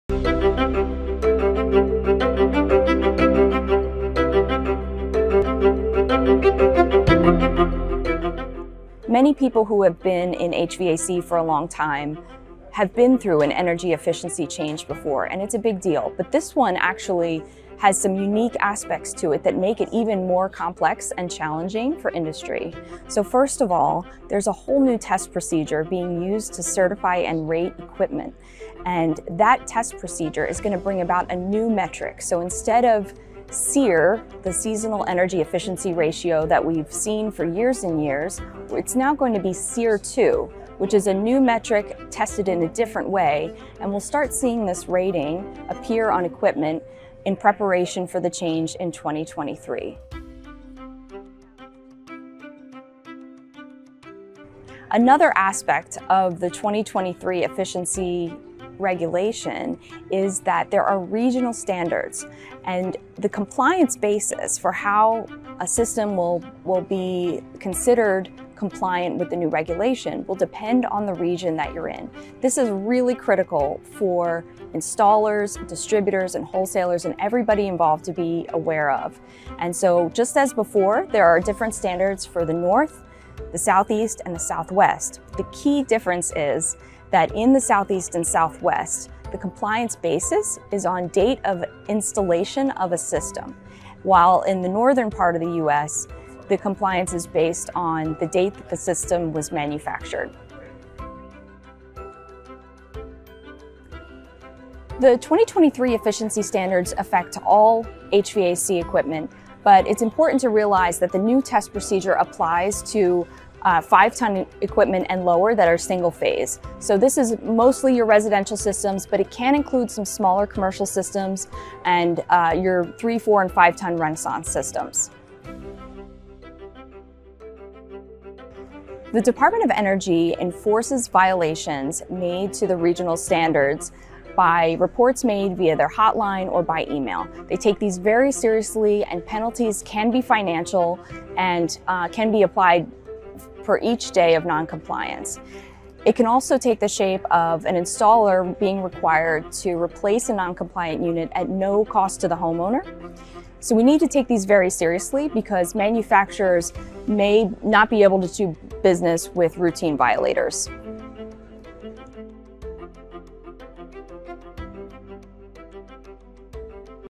SureComfort®: 2023 Energy Efficiency Regulatory Update
Many people involved in the HVAC industry have been through an energy efficiency change before. But this 2023 regulatory update has some unique aspects that make it even more complex and challenging.

Watch the video to learn more about the upcoming changes from Rheem’s Senior Manager of Regulatory Affairs, Allison Skidd, and visit SureComfort.com/HVACKnowZone for additional information and resources.